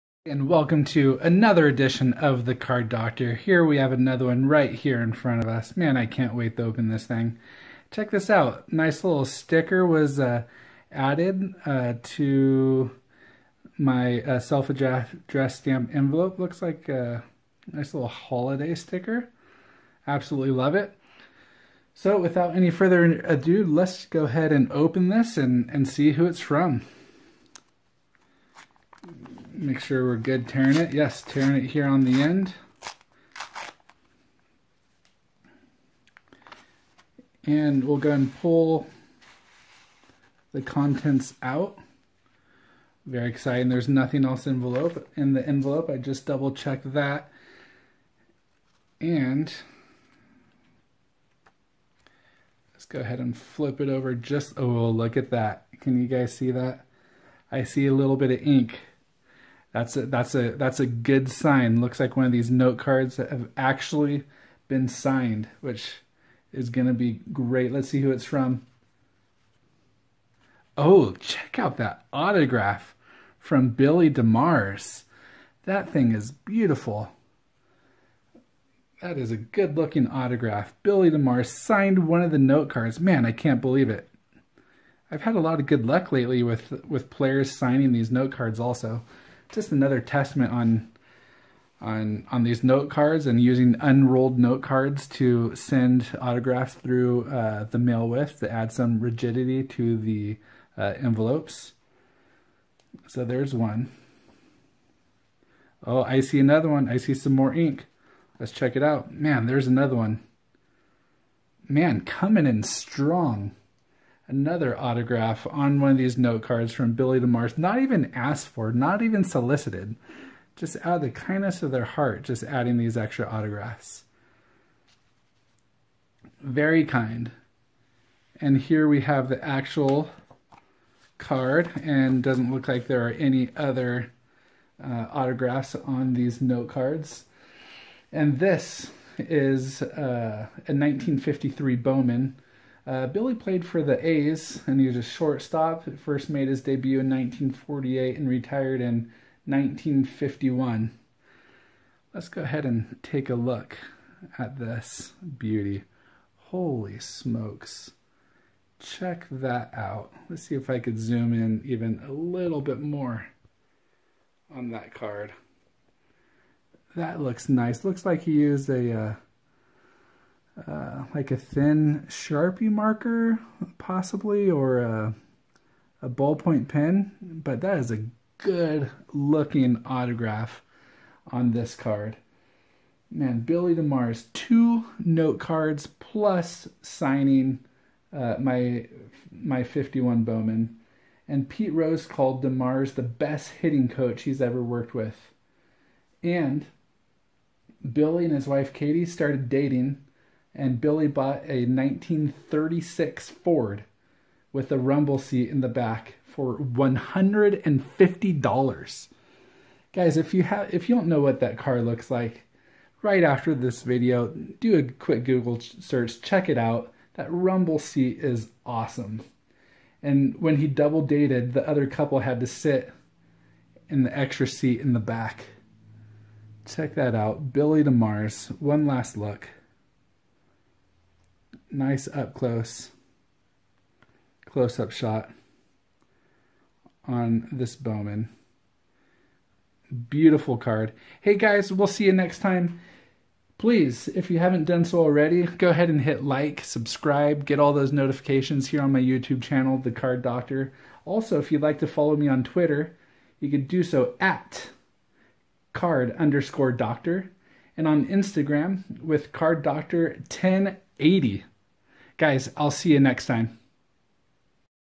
St. Louis Browns Shortstop, TTM Autograph Success
I received a HUGE autograph return from St. Louis Browns sensation! Such an elegant auto!

Are you interested in learning how to send cards and balls through the mail to get autographed?